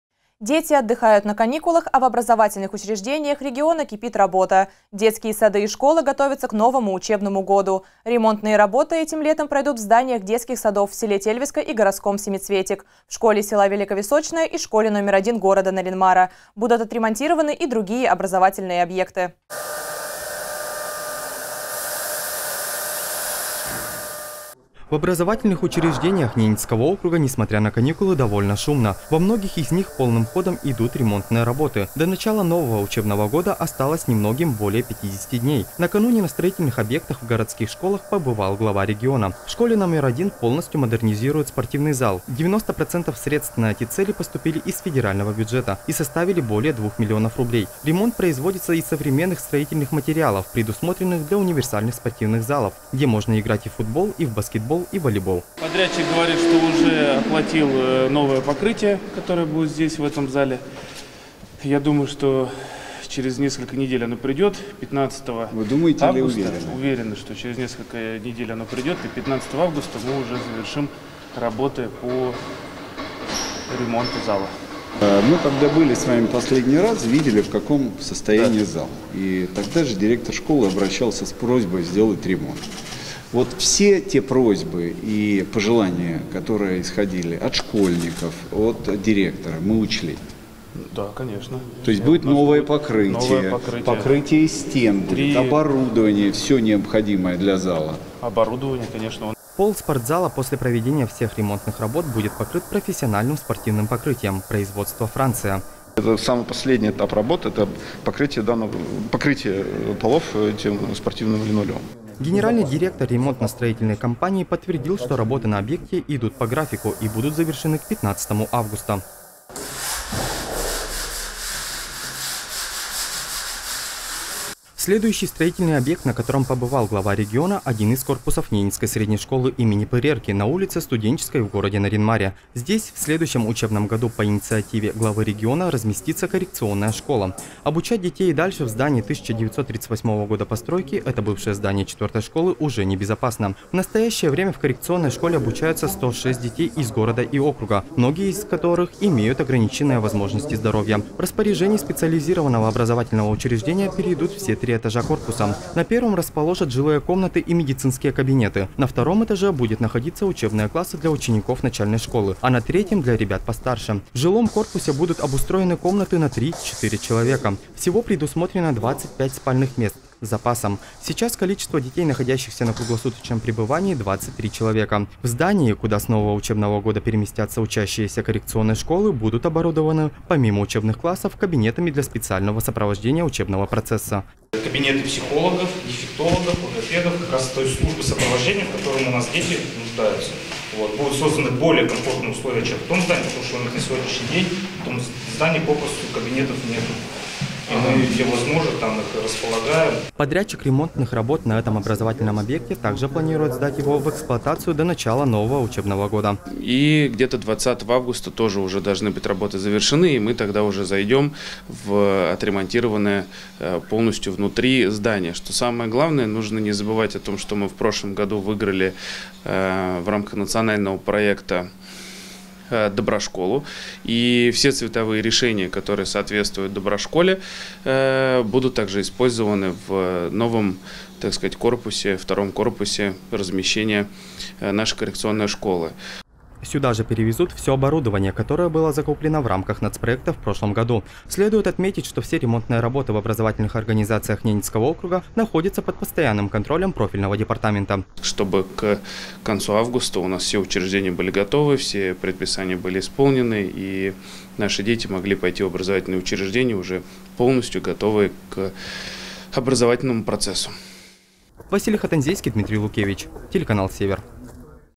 К НОВОМУ УЧЕБНОМУ ГОДУ
Дети отдыхают на каникулах, а в образовательных учреждения региона кипит работа. Детские сады и школы готовятся к новому учебному году. Ремонтные работы этим летом пройдут в зданиях детских садов в селе Тельвиска и городском "Семицветик", в школе села Великовисочное и школе №1 города Нарьян-Мара. Будут отремонтированы и другие образовательные объекты.